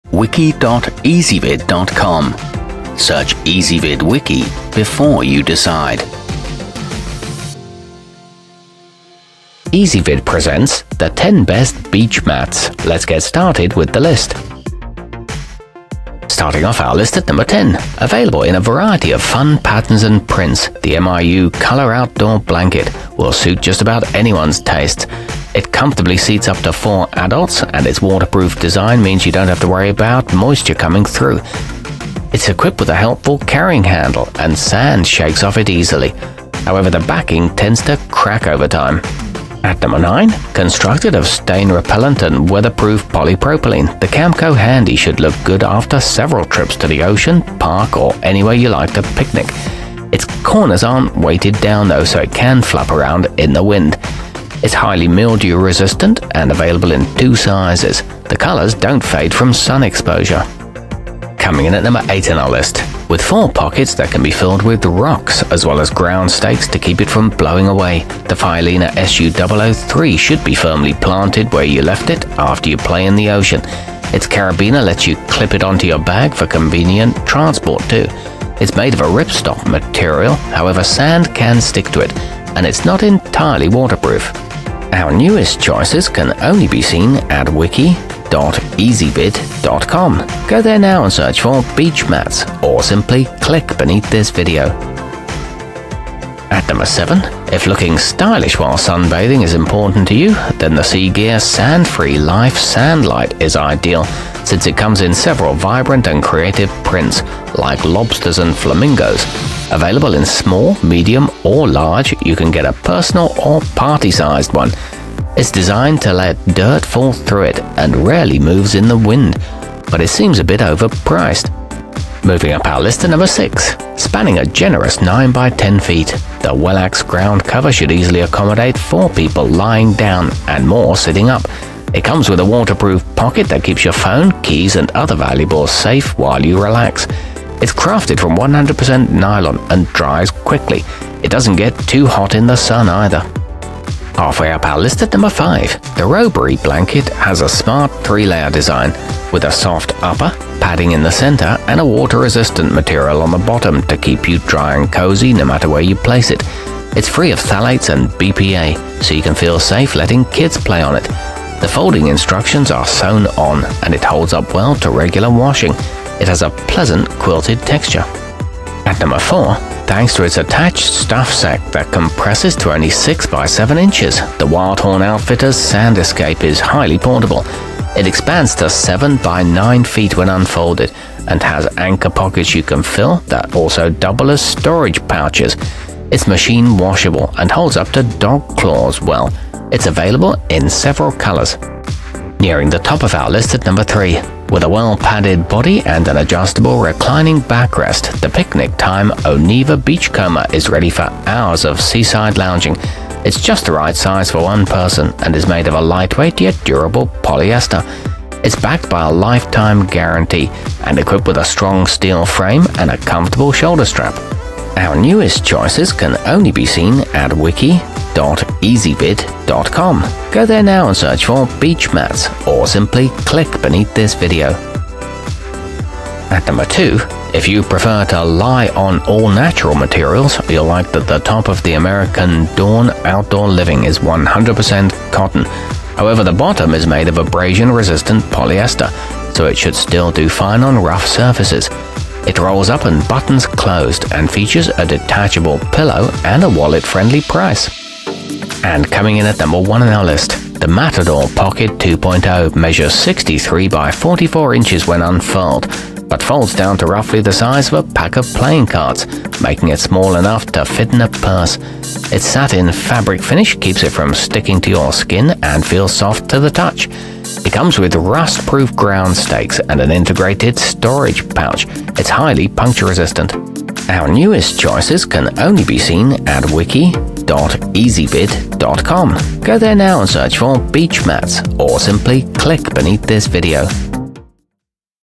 10 Best Beach Mats 2020
Our complete review, including our selection for the year's best beach mat, is exclusively available on Ezvid Wiki.

Beach mats included in this wiki include the american dawn outdoor living, matador pocket 2.0, wildhorn outfitters sand escape, roebury blanket, camco handy, picnic time oniva beachcomber, fylina su003, miu color outdoor blanket, wellax ground cover, and cgear sand-free life sandlite.